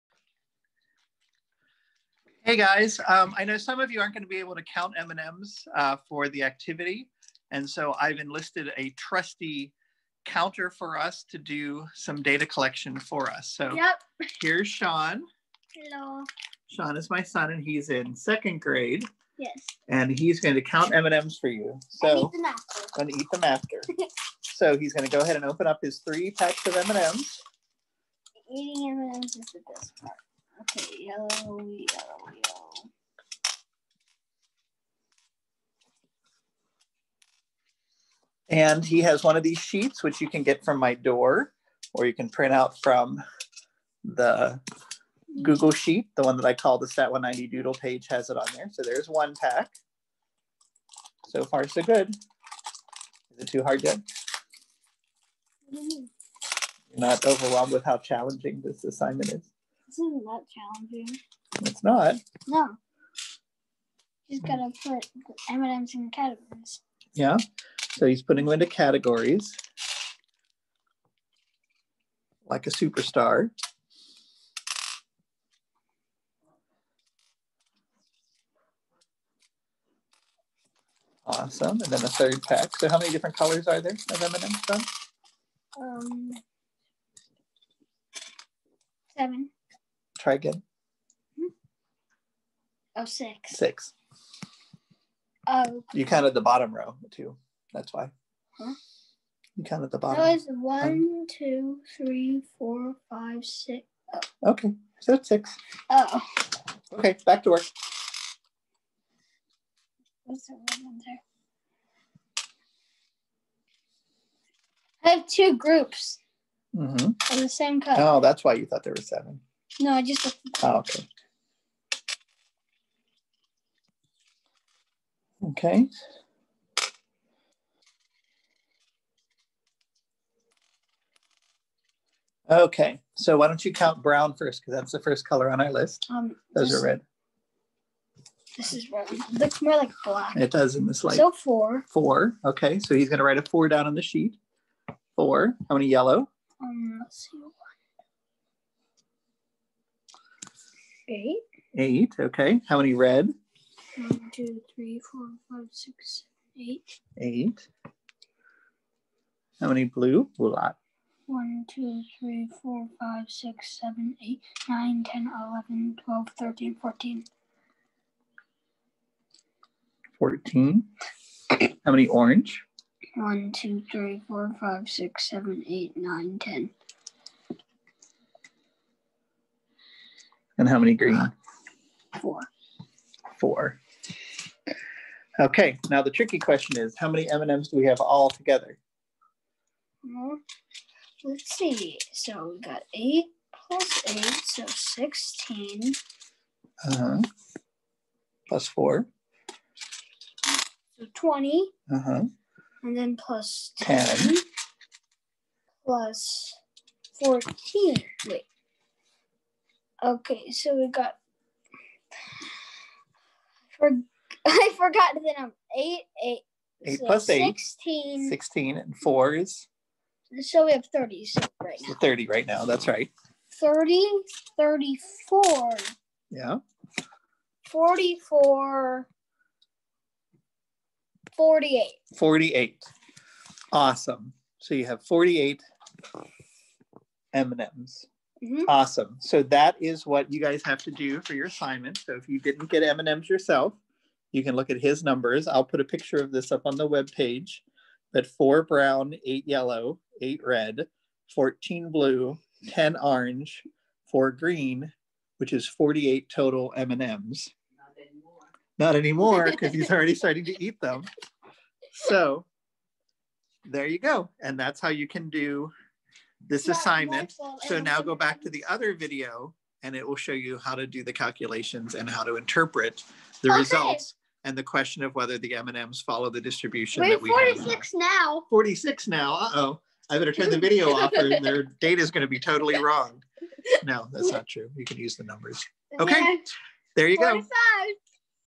Stat 190, Chapter 12 Bonus Video (Guest M&M Counting)
This video provides some data for those who cannot collect their own M&M counts.
It was more useful in 2020, when the pandemic made these kinds of activities impossible, but it's still pretty fun even now.

His final count of 48 total M&MS was:
4 Brown, 8 yellow, 8 red, 14 blue, 10 orange, 4 green

This is an activity video for STAT 190 Introductory Statistics as taught at Truman State University, following the textbook Fundamentals of Statistics, by Sullivan.